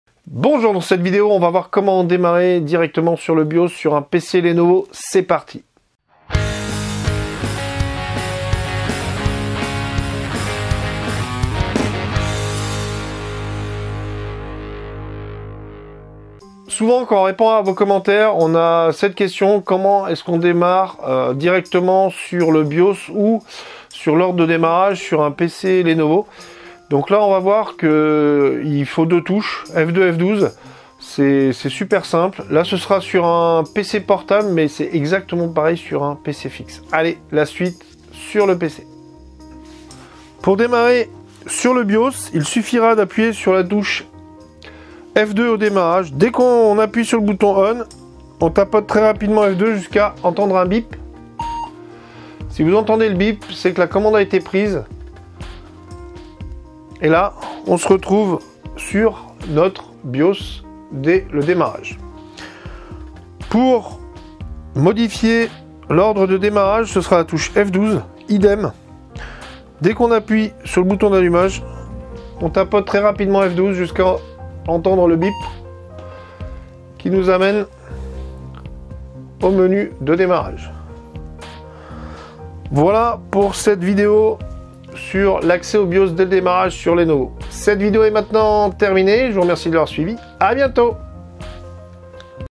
Comment accéder au BIOS et à l'ordre de démarrage boot order sur un PC Lenovo
Dans cette vidéo, nous allons voir comment accéder au BIOS et à l'ordre de démarrage sur un PC Lenovo, qu'il soit portable ou fixe.

Chronologie de la vidéo :
0:00 Générique
0:16 Introduction
0:40 Accéder au BIOS
1:03 Accéder à l'ordre de démarrage
1:22 Conclusion